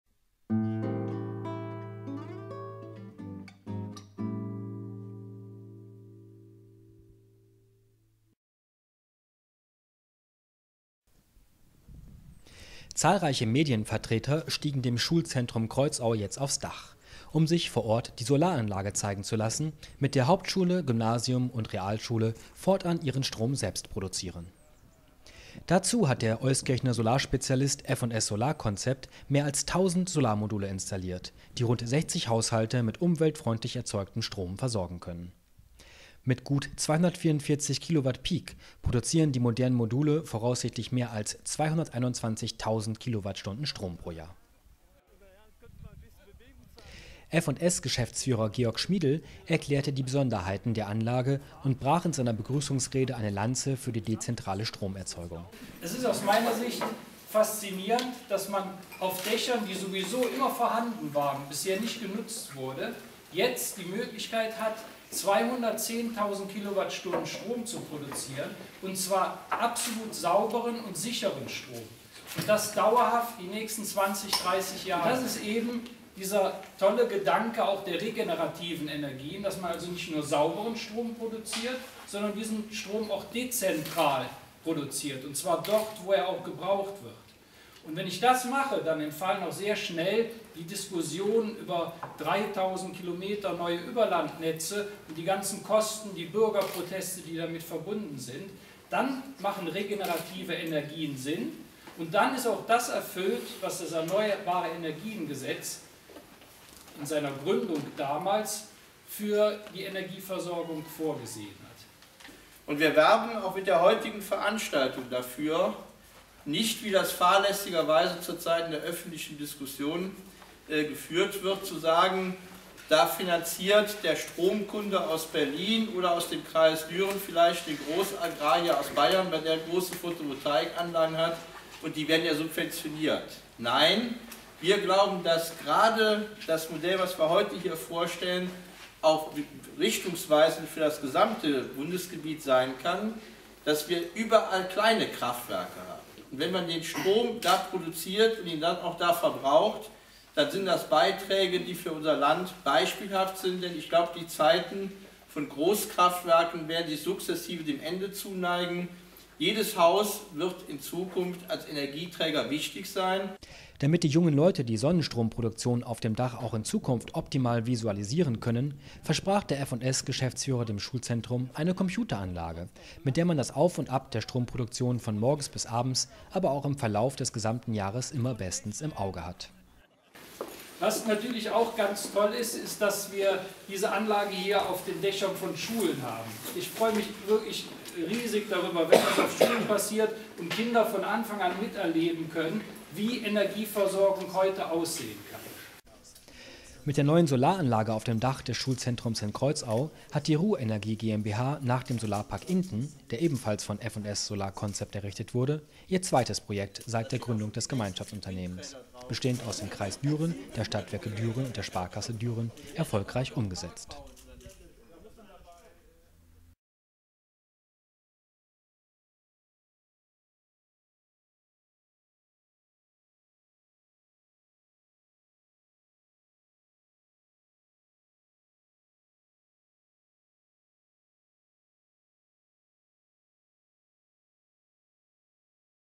Dezentrale Energieversorgung am Schulzentrum Kreuzau
Das Schulzentrum Kreuzau produziert seinen Strom fortan selbst. Auf den Dächern von Gymnasium, Realschule und Hauptschule hat der Euskirchener Solarspezialist „F&S solar" mehr als 1000 Solarmodule installiert, die rund 60 Haushalte mit umweltfreundlich erzeugtem Strom versorgen können. „Mit gut 244,4 Kilowattpeak produzieren die modernen Module voraussichtlich mehr als 221.000 Kilowattstunden Strom pro Jahr", so F&S-Geschäftsführer Georg Schmiedel.
„Es ist für mich faszinierend, dass man auf Dächern, die bislang nicht genutzt wurden, jetzt die Möglichkeit hat, absolut sicheren und sauberen Strom zu produzieren und das die nächsten 30 Jahre lang", sagte er anlässlich einer Vertragsunterzeichnung, die das Solarkraftwerk am Montagnachmittag in die Hände der Rurenergie legte.
Das Besondere an der Photovoltaik-Anlage sei, so Schmiedel, dass der Strom zu einer Tageszeit produziert werde, da der Betrieb in der Schule auf Hochtouren laufe. „Da liegt es natürlich nahe, dass der Strom, der auf dem Dach produziert wird, auch direkt in der Schule verbraucht wird", so Schmiedel, der in seiner Rede vor allem eine Lanze für die dezentrale Stromerzeugung brach. Dezentrale Stromversorgung bedeute unter anderem auch, dass man sich die Diskussionen um neue Überlandnetze sparen könne und damit auch die Kosten und Bürgerproteste. Nur in ihrer dezentralen Realisierung seien erneuerbare Energien sinnvoll.
Schmiedel freute sich besonders darüber, dass die Photovoltaik-Anlage auf einer Schule gebaut wurde. „Es ist sehr gut, wenn Schülerinnen und Schüler vor Ort erleben können, wie Energieversorgung heute aussehen kann." Und damit die jungen Leute die Sonnenstromproduktion auf dem Dach auch in Zukunft optimal visualisieren können, versprach der F&S-Geschäftsführer dem Schulzentrum eine Computeranlage, mit der man das Auf und Ab der Stromproduktion von morgens bis abends, aber auch im Verlauf des gesamten Jahres immer bestens im Auge habe.
Mit der neuen Solaranlage auf dem Dach des Schulzentrums in Kreuzau hat die Rurenergie GmbH nach dem Solarpark Inden, der ebenfalls von „F&S solar" errichtet wurde, ihr zweites Projekt seit der Gründung des Gemeinschaftsunternehmens - bestehend aus dem Kreis Düren, der Stadtwerke Düren und der Sparkasse Düren - umgesetzt. „Mit dieser Anlage werden jährlich allein 125 Tonnen CO2 eingespart", so Heinrich Klocke, Geschäftsführer der Stadtwerke Düren. Allein diese Tatsache sei bereits die Investition von 480.000 Euro wert gewesen. Dirk Hürtgen, Geschäftsführer der Rurenergie, fügte hinzu, dass die Solarmodule auch dann noch umweltfreundlich seien, wenn sie nach vielen Jahren wieder von den Schuldächern abmontiert werden müssten. „Denn die Silizium-Module sind recyclebar und belasten so nicht zukünftige Generationen."
Auch die drei Schulleiter freuten sich über ihr neues Solardach, dass fortan nicht nur Thema im Physikunterricht, sondern auch in der Sozialwissenschaft sein werde. „Damit leisten wir nicht nur einen Beitrag zum Umweltschutz, sondern lassen erneuerbare Energien für unsere Schüler auch direkt erlebbar werden", so Schulleiter Wolfgang Röther.
Landrat Wolfgang Spelthahn erklärte, dass die Anlage auf dem Schuldach ein weiterer Schritt zum Ziel sei, den Kreis Düren in „nicht allzu ferner Zukunft" einmal allein durch erneuerbare Energien mit Strom zu versorgen. Spelthahn pflichtete Schmiedel bei, dass es wichtig sei, auf viele Einzelprojekte zu setzen und damit auf die dezentrale Stromgewinnung direkt vor Ort. Er bedankte sich beim F&S-Geschäftsführer vor allem für die zugesagte Computeranlage, die es ermögliche, den Schülerinnen und Schülern vor allem die „Sinnhaftigkeit vor Ort produzierter Energie" deutlich werden zu lassen. Junge Leute über die Möglichkeiten regenerativer Energien aufzuklären, sei eine „entscheidende Weichenstellung für die Zukunft", so der Landrat.